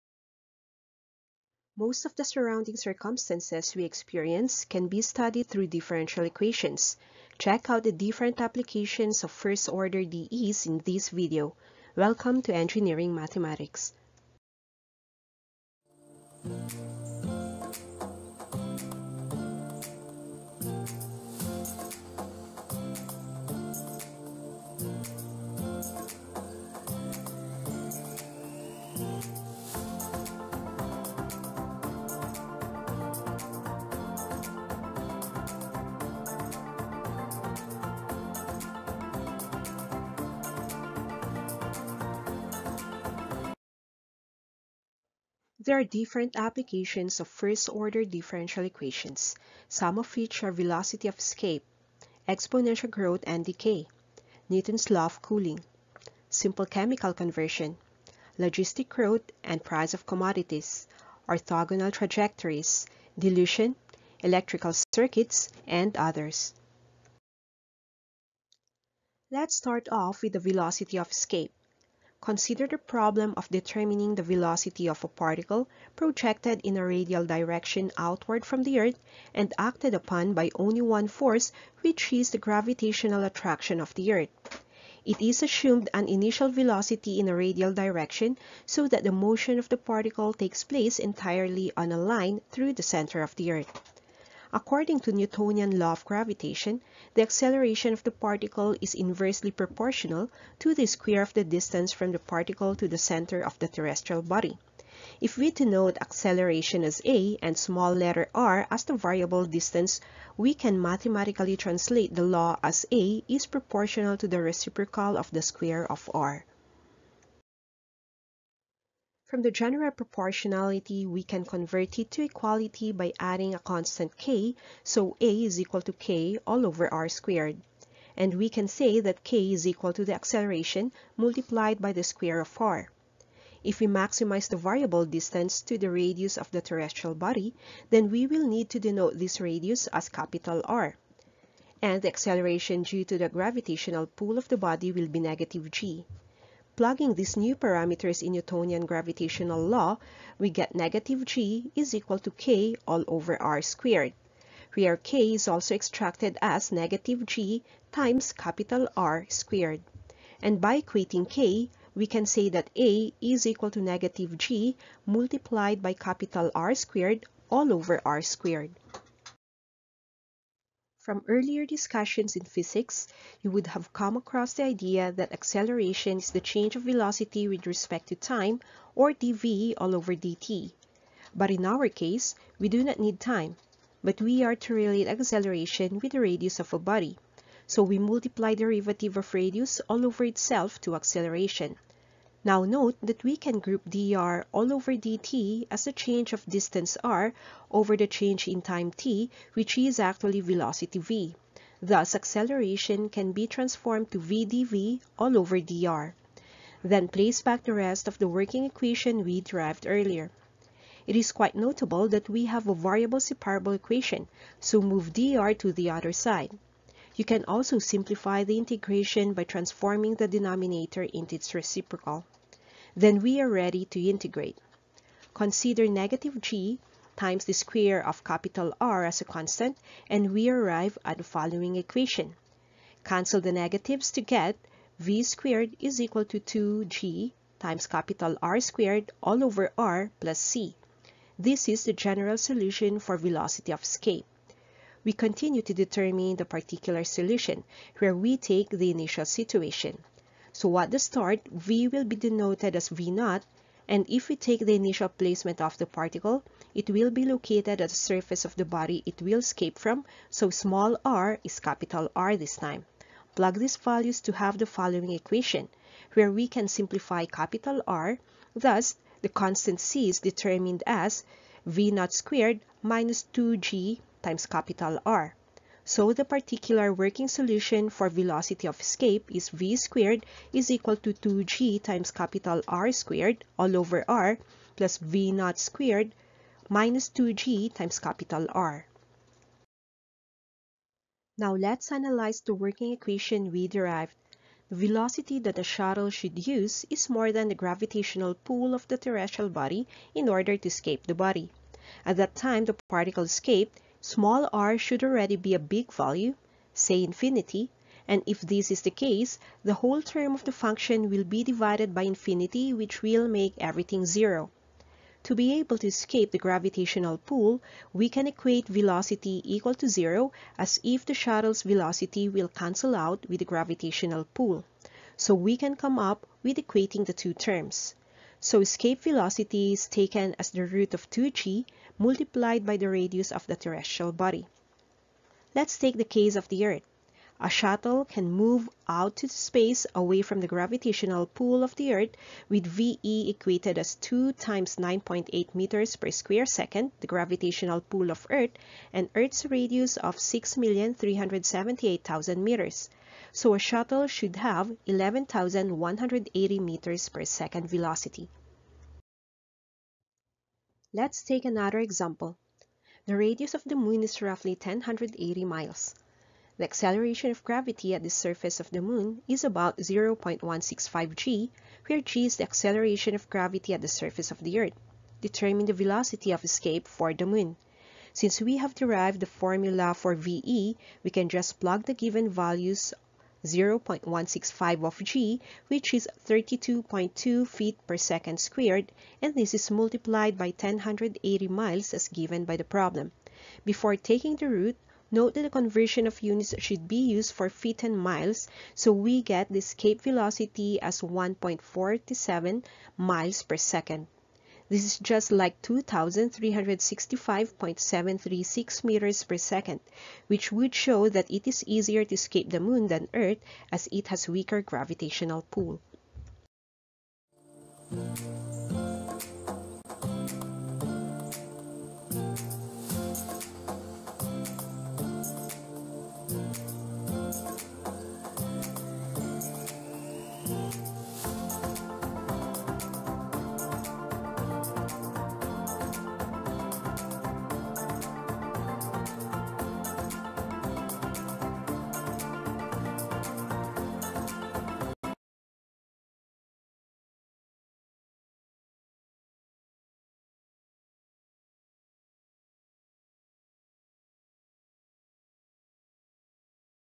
DIFFERENTIAL EQUATIONS - 5A: DE APPLICATIONS: ESCAPE VELOCITY
Escape velocity is an application of first-order DE. Learn the derivation here.